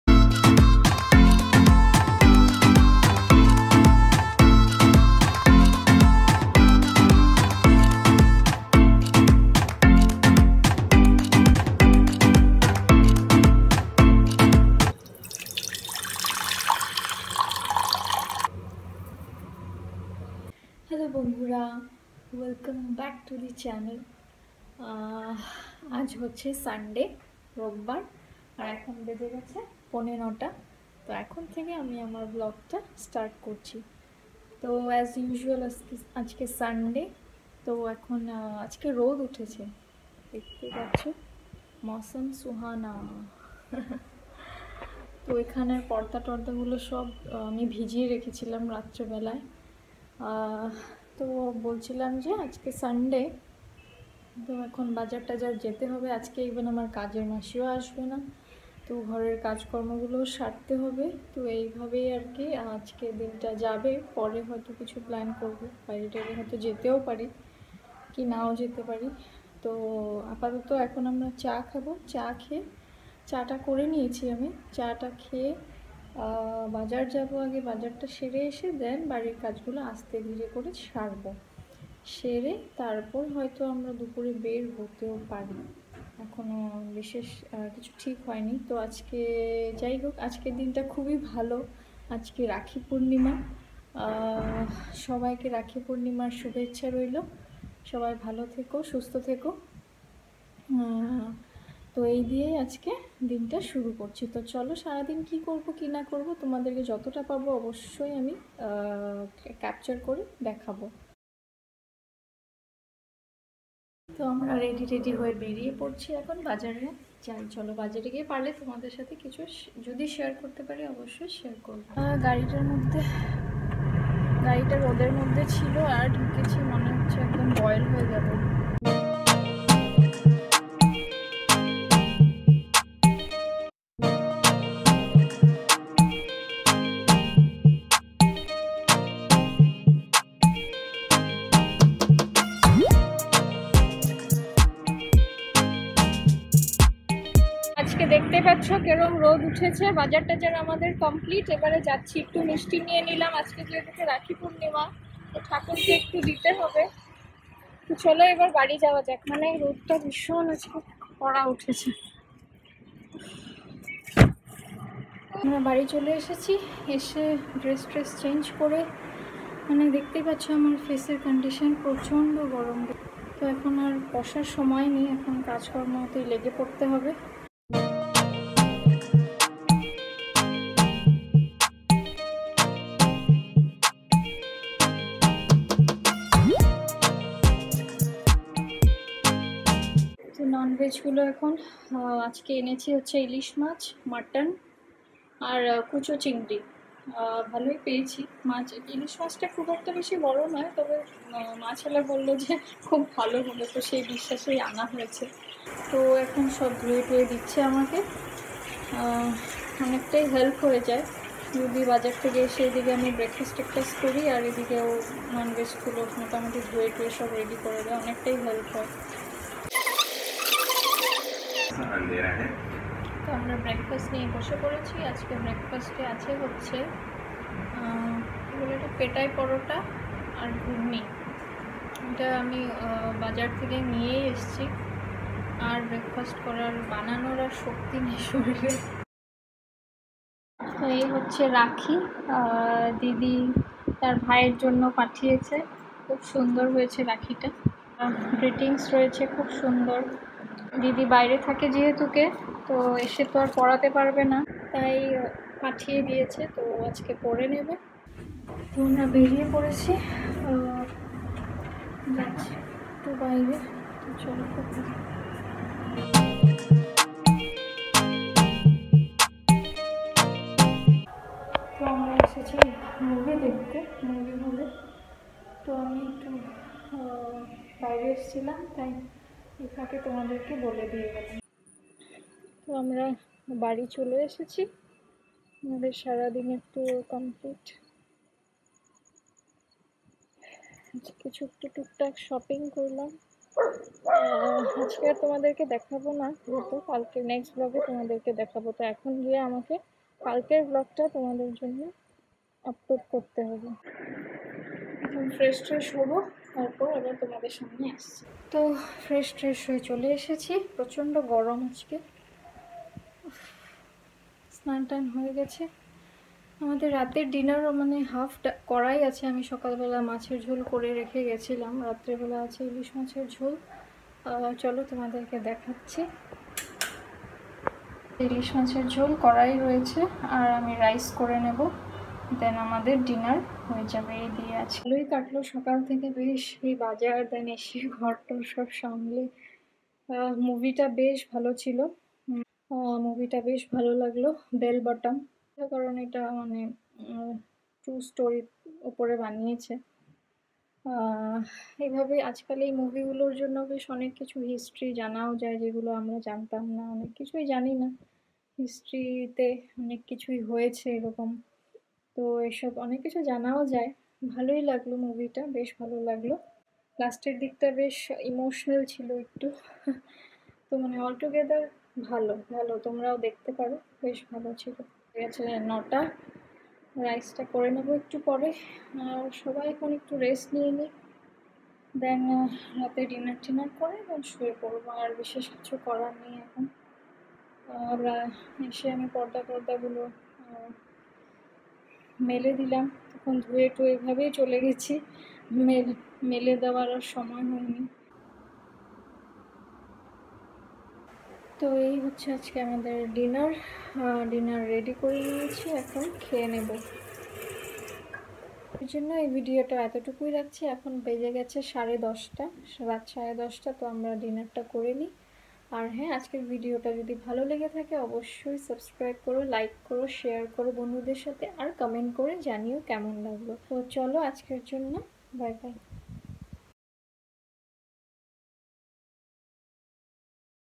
Hothat Plan Kore Beriye Porlam | Poore Gelo Shorir | Illish Diye Jomiye Bhuribhoj Holo | Vlog 7
Hothat Plan Kore Beriye Porlam | Poore Gelo Shorir | Illish Diye Jomiye Bhuribhoj Holo | Indian Vlogger Koyal